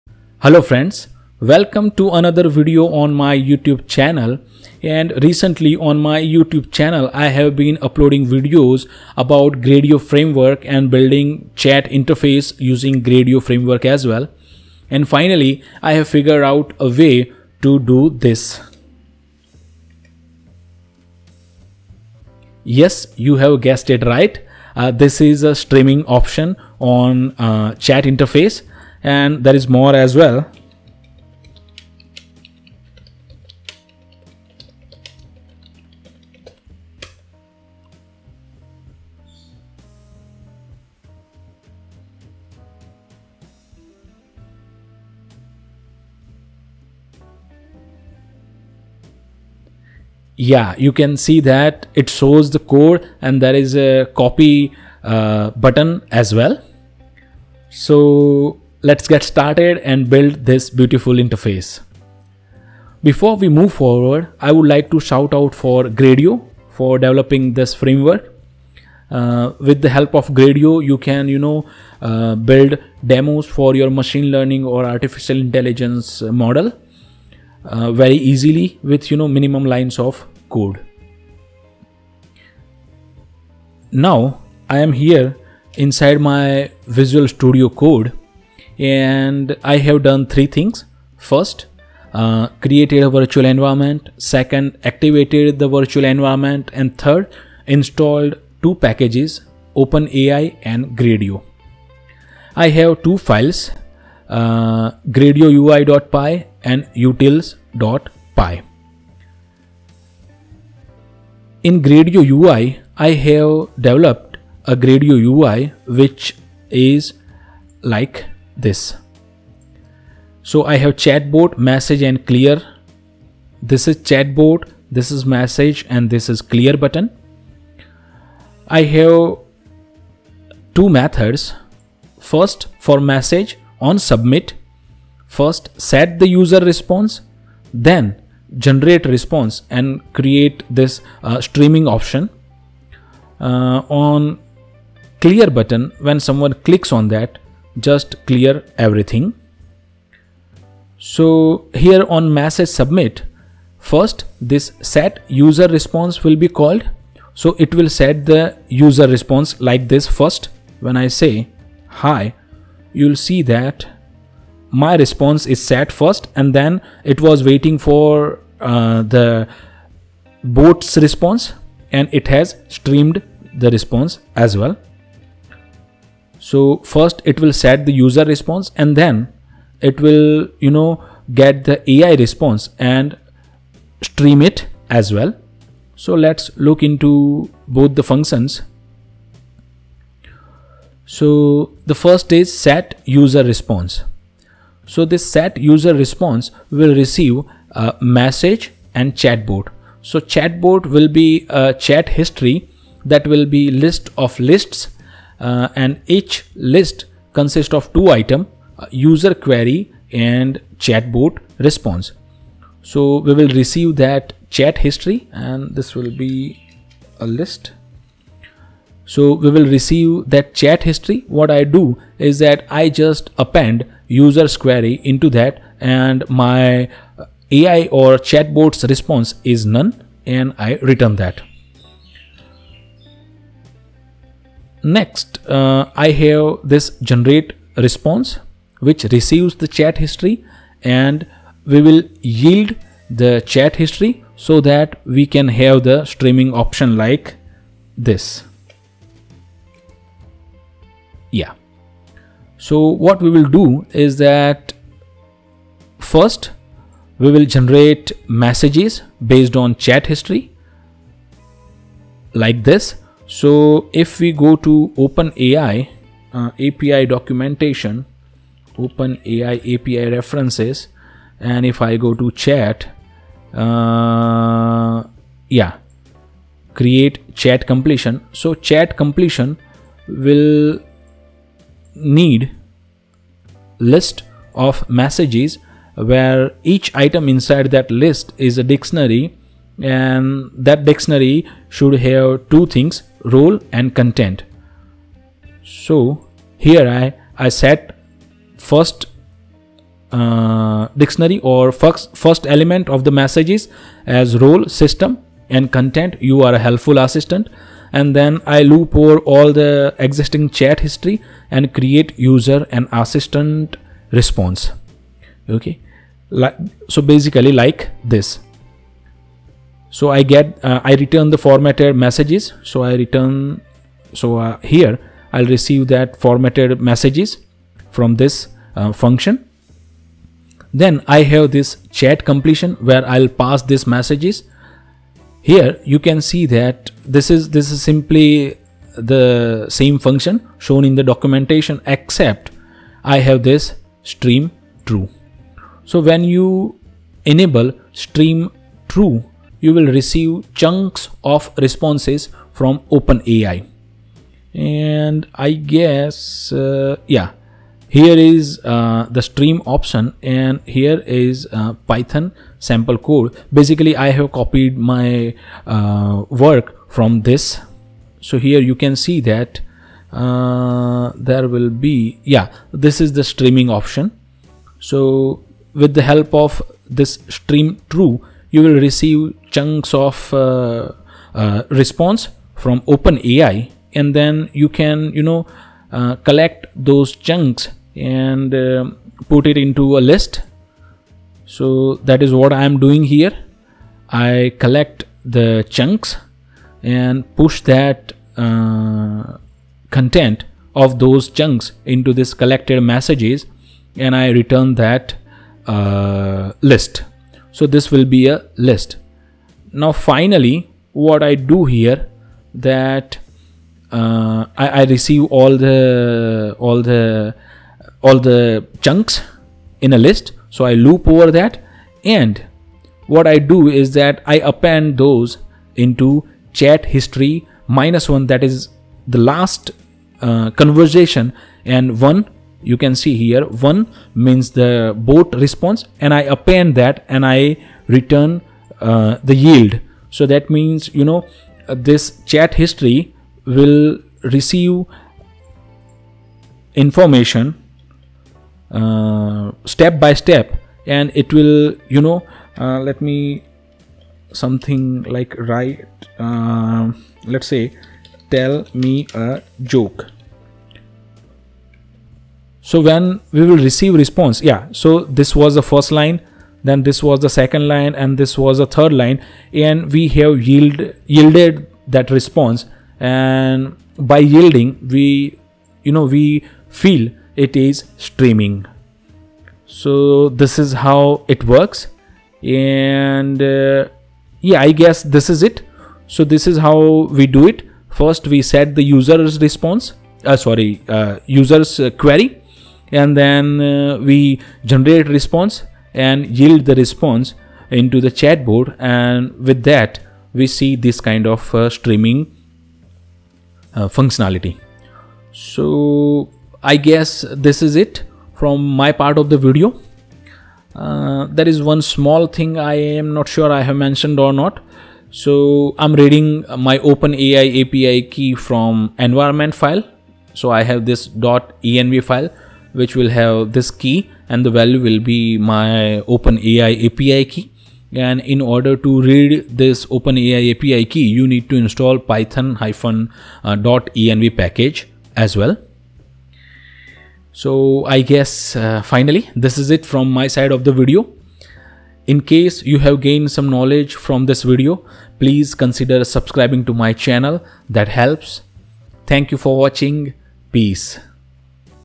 Streaming on chat interface using Gradio framework | Openai | Python | Chat Web UI
Learn to build a streaming chatbot using Openai, Python, and Gradio in just a few lines of code.

You will learn the following things:
(1) Understand Openai API streaming responses
(2) Build a chat interface for streaming
(3) Connect all the pieces together and build a streaming chatbot

Learn to build this streaming chatbot with me in a live session

Peace.